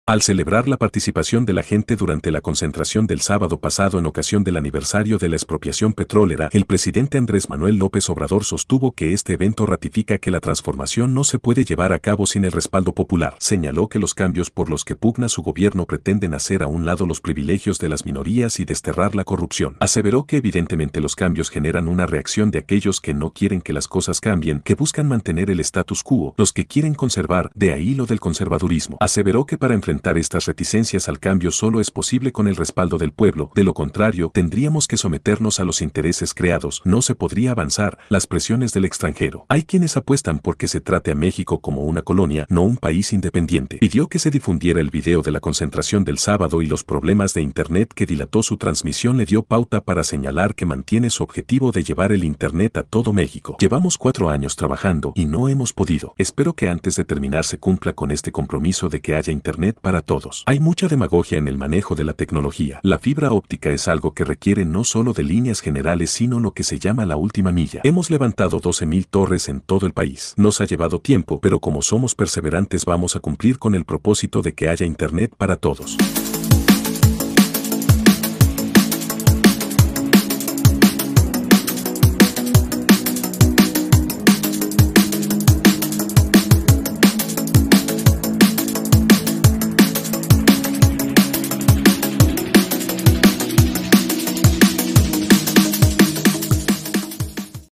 Sin apoyo del pueblo no se puede gobernar correctamente : AMLO| agradeció asistencia al zócalo cdmx
DONACIONES VOLUNTARIAS AL CANAL :  # CUENTA BANCOMER : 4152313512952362